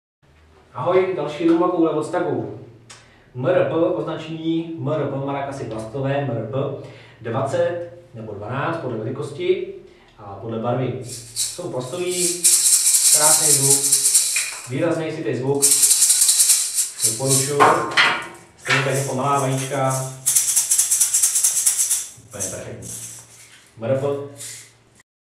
Plastové maracasy Stagg MRP-20YW a Stagg MRP-12PK
Pár plastových maracas.

Žluté se zelenými prvky, 20 cm.

Růžovo-oranžové, 12 cm.